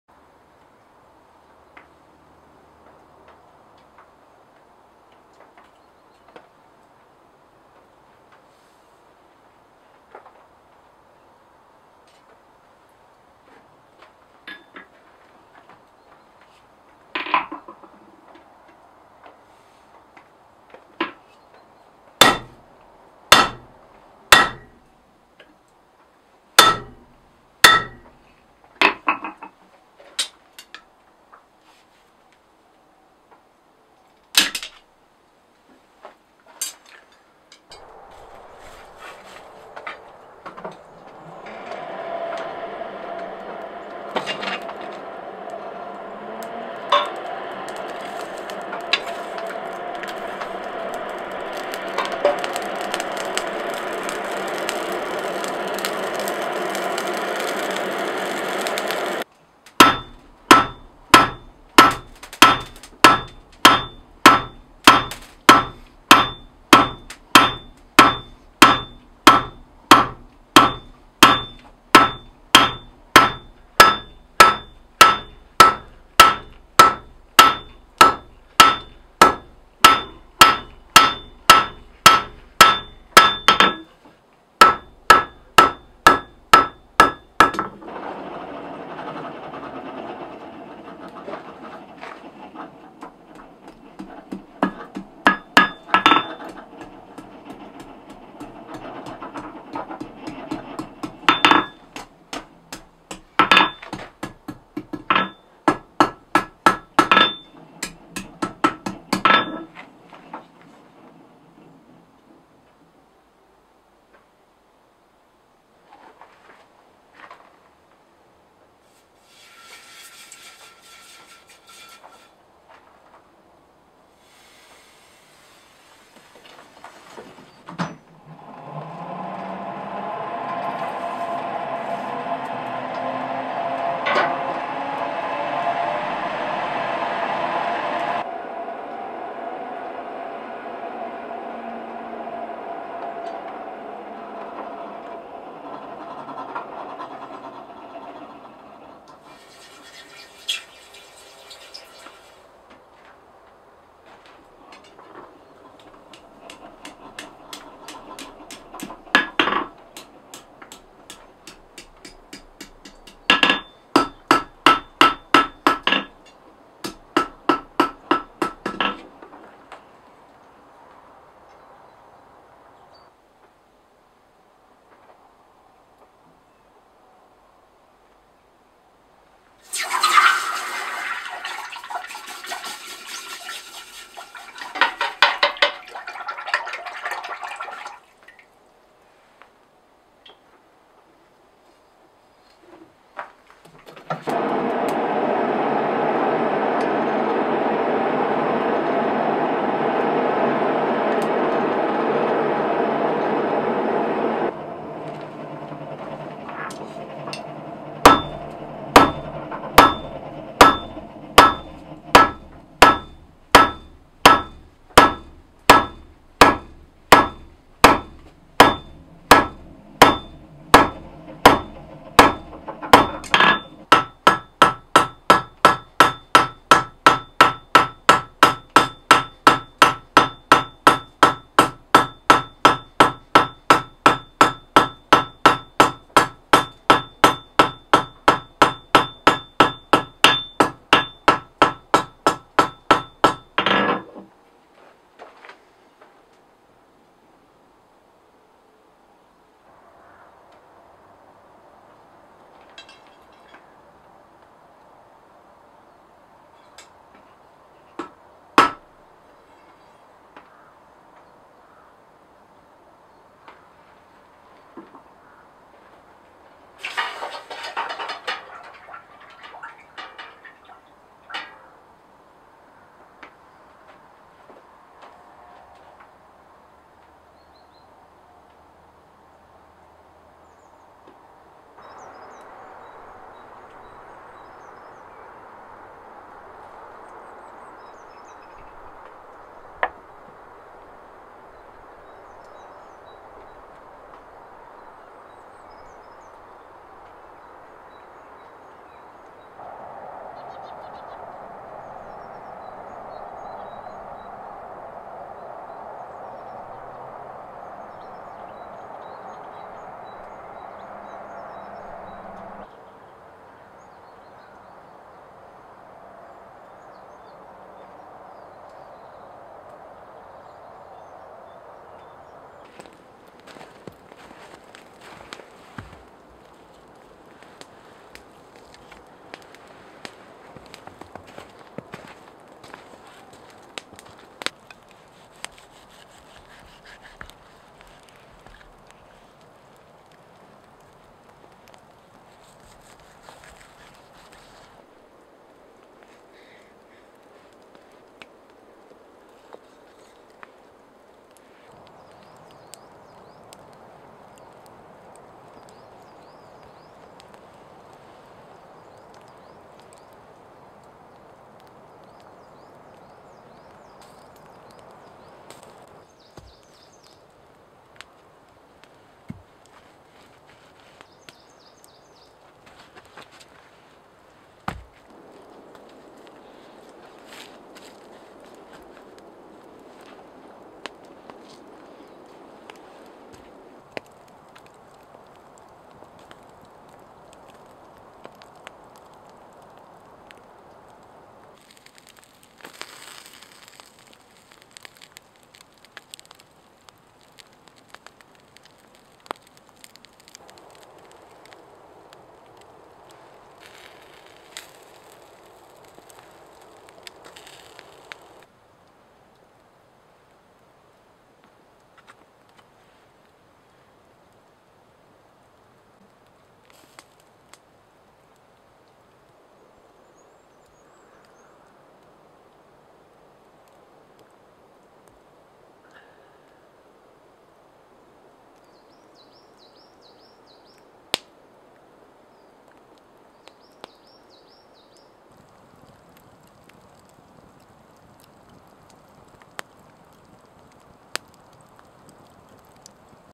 Blacksmithing: making grill spear
I hand forge a spear in my traditional blacksmith shop and grill some meat in the forrest. I use mild steel about 6mm.

Location: Denmark.

Video gear: Nikon D7000, Nikon 50 1.8, Røde videomacro, iMovie.